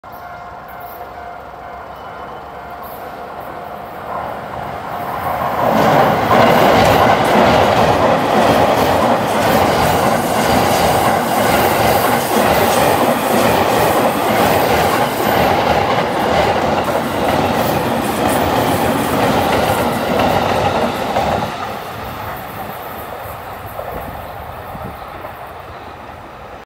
上野東京ラインE231系15両 赤羽〜尾久間踏切通過
上野東京ライン　赤羽〜尾久　にて撮影